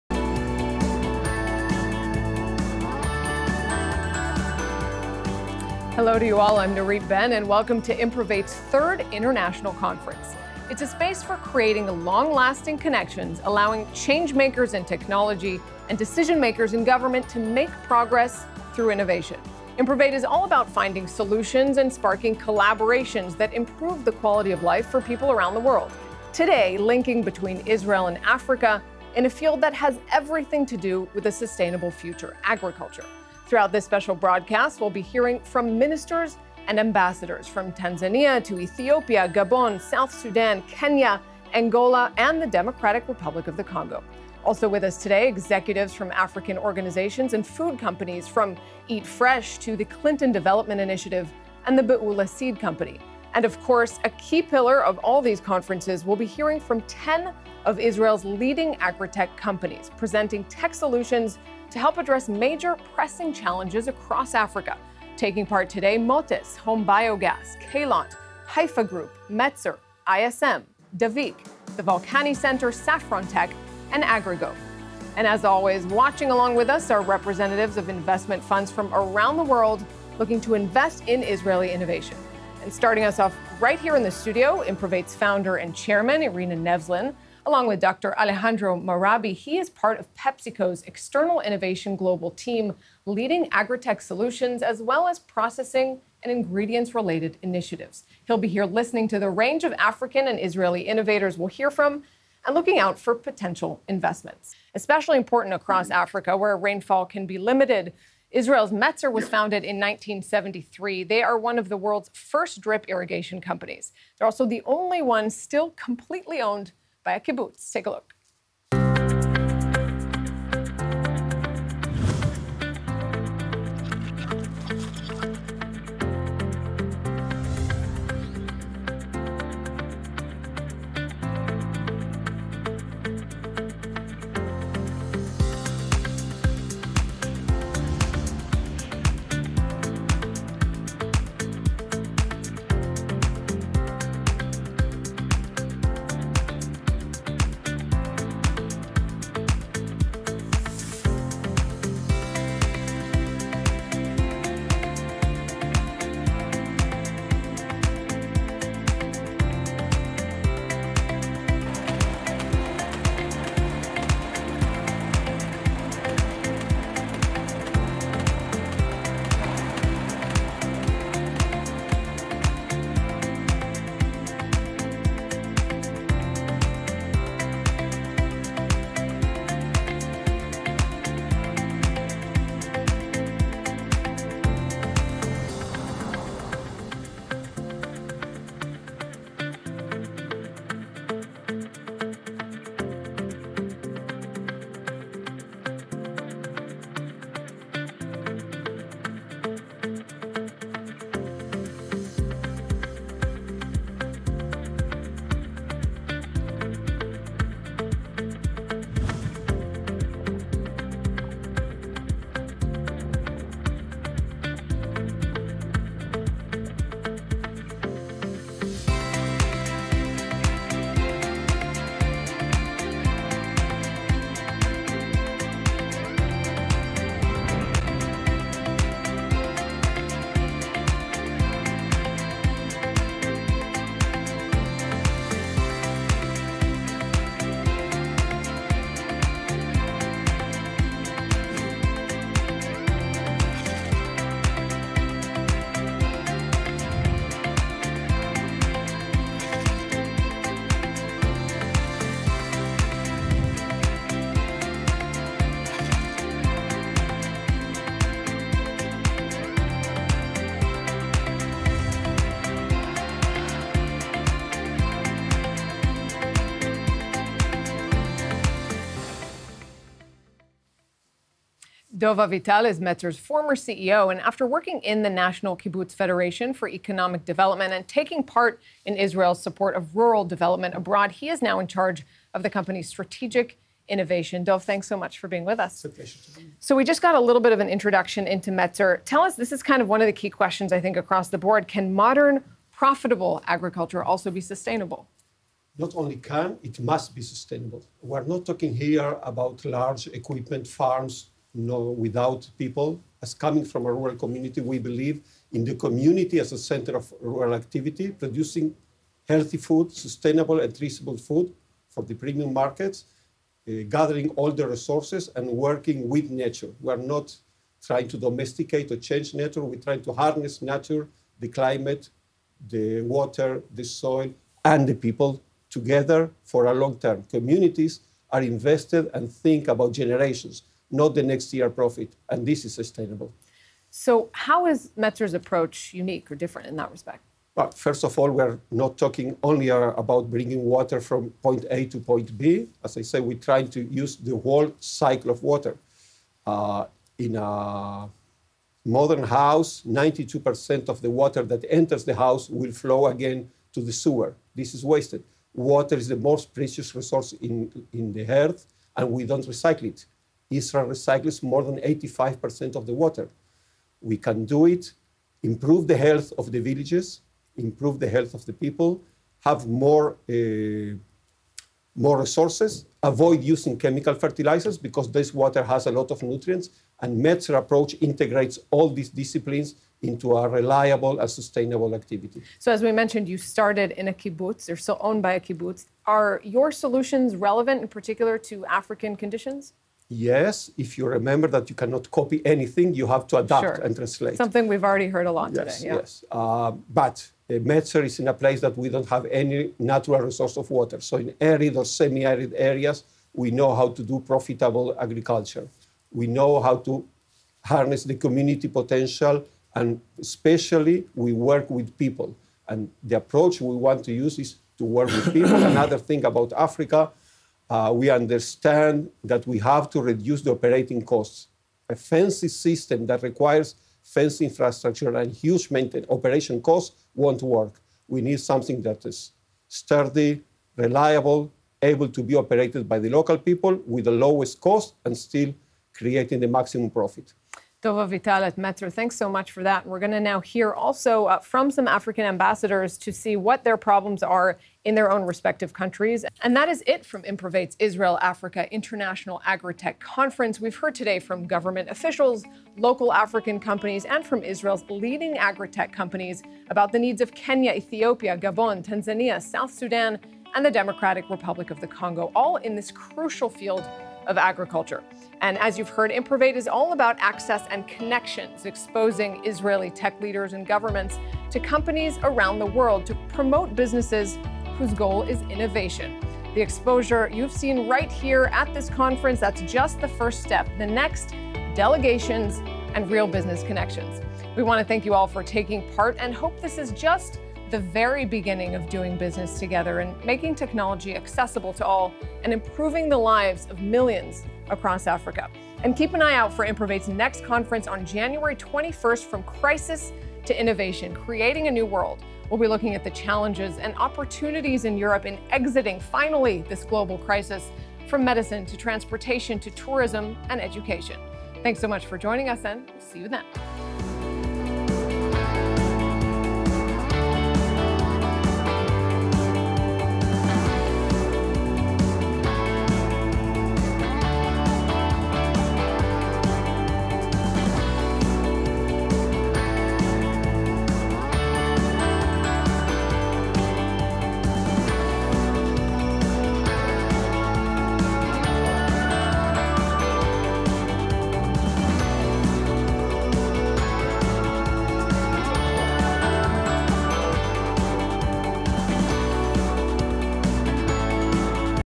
Metzer showcase at Improvate 2020 agri-tech conference
See Metzer’s strategy and innovation manager, Dov Avital, discuss Metzer’s unique irrigation technologies at Improvate’s 2020 agri-tech conference, focused on unique Israeli solutions to agricultural development challenges in Africa.